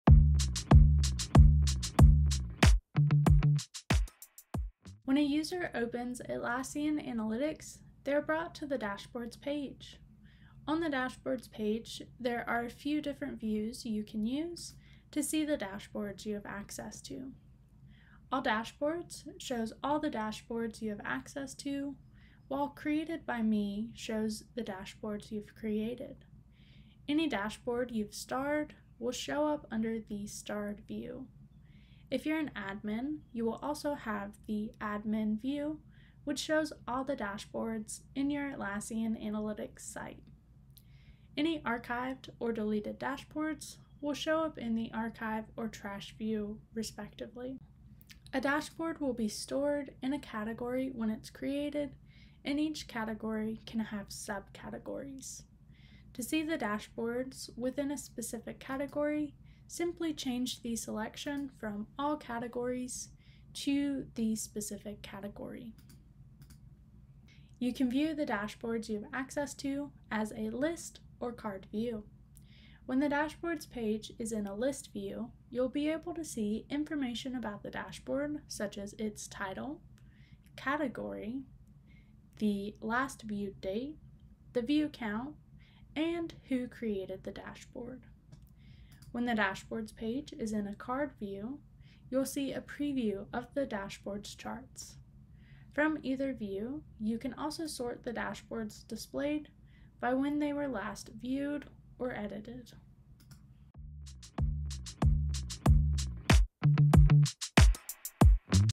Dashboard Homepage | Atlassian Analytics - Demos
Watch this demo to learn how to get started with the dashboard homepage in Atlassian Analytics

To ensure we’re providing useful content, please let us know if this video is helpful by liking it.